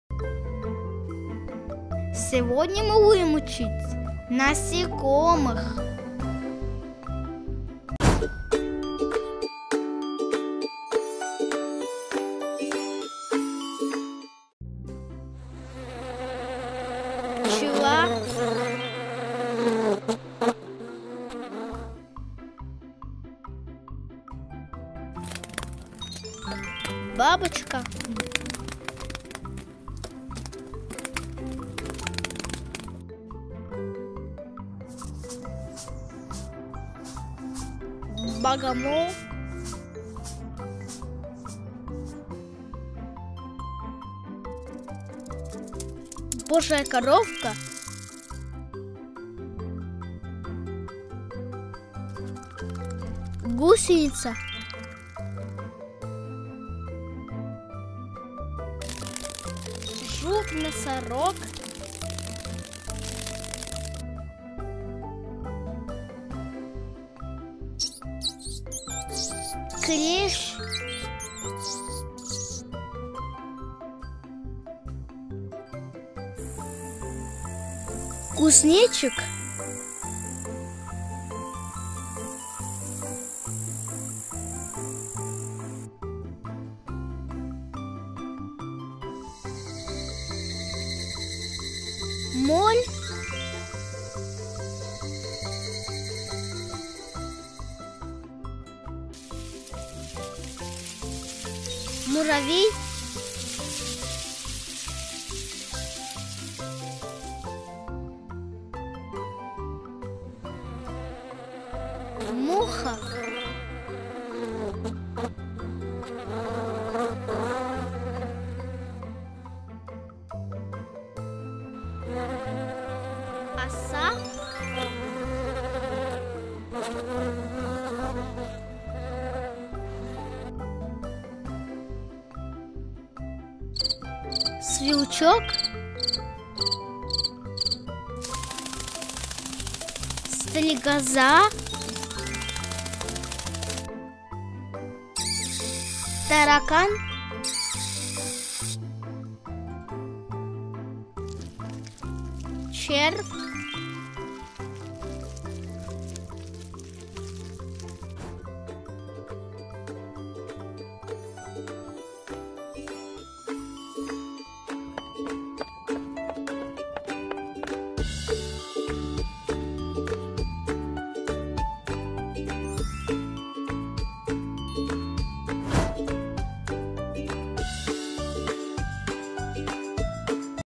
Изучаем Животных - Насекомые!🐞 | Учим названия и виды насекомых. Развивающие Детские Мультики
Насекомые для детей. Учим названия и виды. Изучаем Животных - Насекомые!🐞 | Учим названия и виды  насекомых. Развивающие Детские Мультики